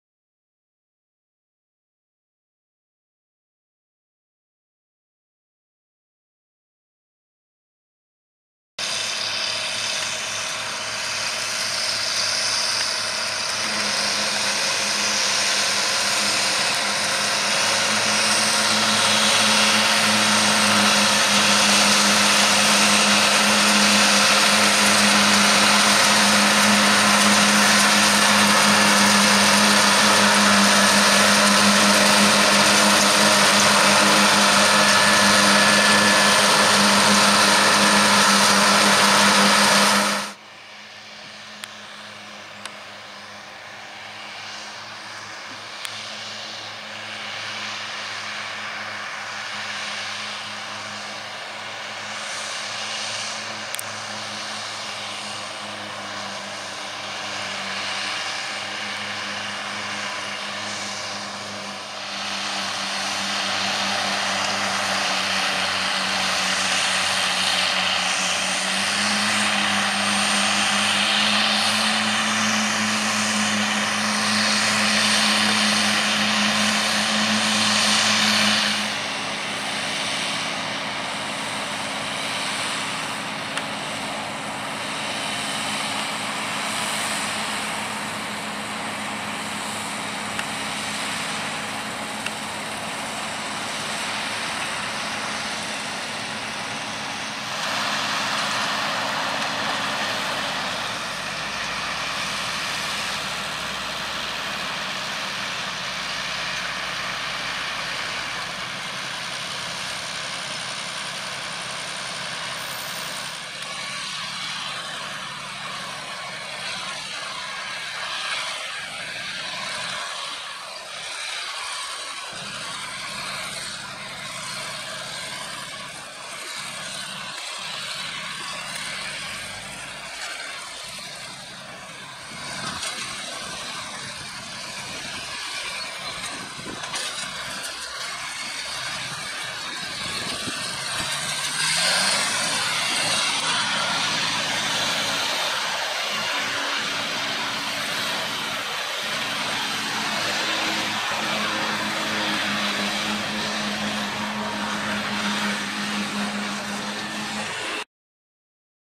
Silage 2020 - Lifting Grass with Reco Mengele on NH 8340 Ford & Massey Ferguson 6490
Todays catch and a lovely classic outfit here on the job of chopping some good 1st cut, not a weed in sight in the ley. A lovely New Holland 8340 Ford on a Reco Mengele SH40N, carting is done with Massey Ferguson 6490 & 6290, the 6290 not in video. Filmed in Hampshire, UK. Watch in HD for best quality.